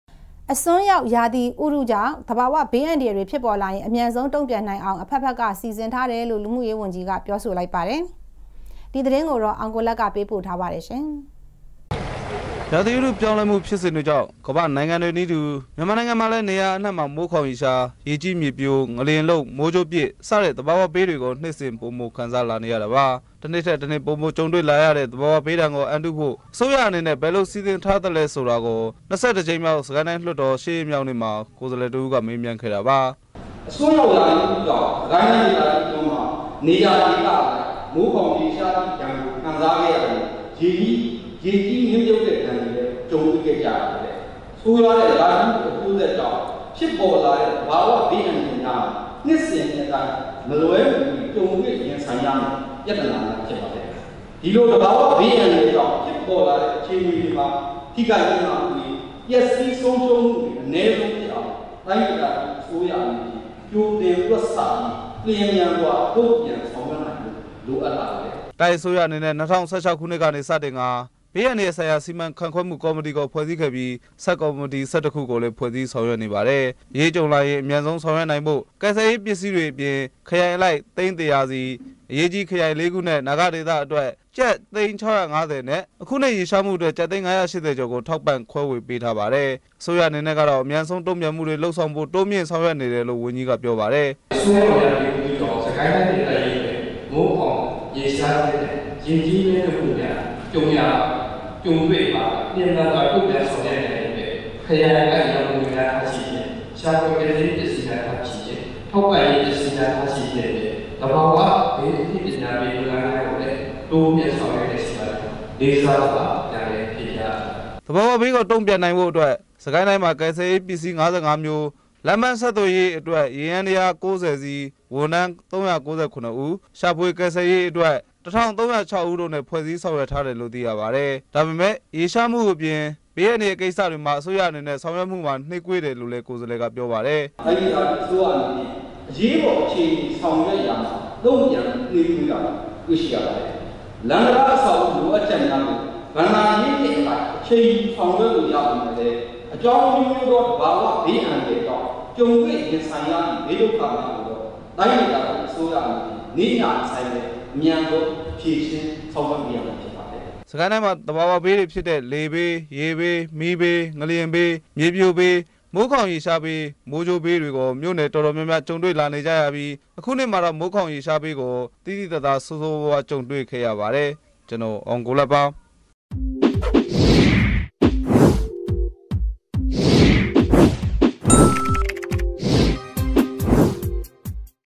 DVB - သဘာဝဘေးအန္တရာယ်တွေဖြစ်ပေါ်လာရင် အမြန်ဆုံး တုံ့ပြန်နိုင်အောင် အဖက်ဖက်ကစီစဉ်ထား
အစွန်းရောက်ရာသီဥတုတွေကြောင့် သဘာဝဘေးအန္တရာယ်တွေဖြစ်ပေါ်လာရင် အမြန်ဆုံး တုံ့ပြန်နိုင်အောင် အဖက်ဖက်က စီစဉ်ထားတယ်လို့ လူမှုရေးဝန်ကြီးက ပြောဆိုလိုက်ပါတယ်။ ဒီသတင်းကို အောင်ကိုလတ်က ပေးပို့ထားပါတယ်
DVB TV -